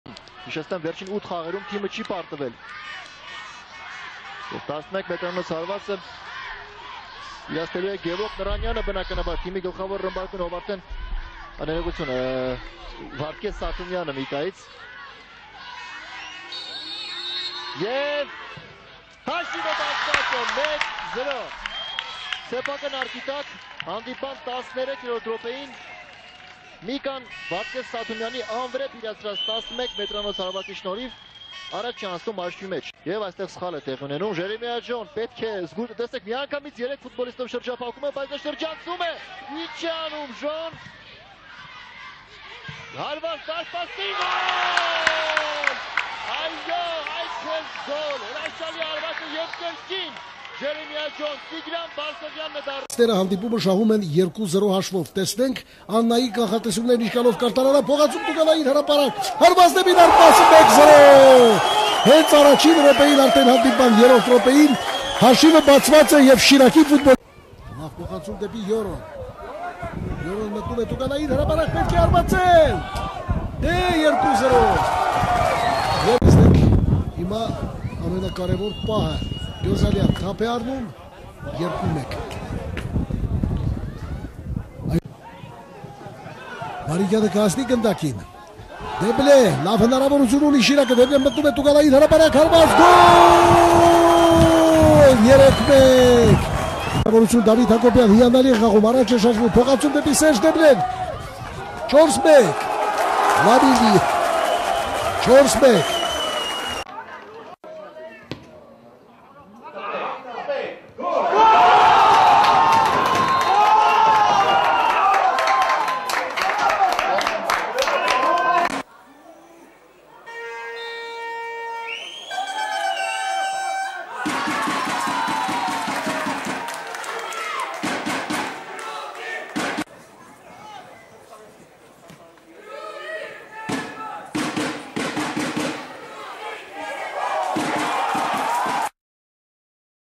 APL. 23 Matchday. All goals
FC Mika-FC Ararat 2:0 (2:0)

Yerevan, Mika stadium

Referee -- Suren Baliyan

Goals -- Vardges Satumyan, 13, 11m, Tigran Barseghyan, 16

FC Shirak-FC Alashkert 4:1 (2:0)

Gyumri,City stadium

Referee -- Zaven Hovhannisyan

Goals -- Tigran Davtyan, 3, Serj Deble, 43, Davit Hakobyan, 52, Lamine Li Yorom 60 (FC Shirak), Norayr Gyozalyan, 47, 11m (FC Alashkert)

Ulisses FC-FC Banants 0:1 (0:0)

Abovyan, City stadium

Referee -- Manuk Fahradyan

Goal -- Hovhannes Hambardzumyan, 90+1

FC Pyunik-FC Gandzasar 1:1 (1:0)

Yerevan, Football academy

Referee -- Gevorg Eghoyan

Goals -- Aghvan Papikyan, 5, 11m (FC Pyunik), Narek Beglaryan, 90 (FC Gandzasar)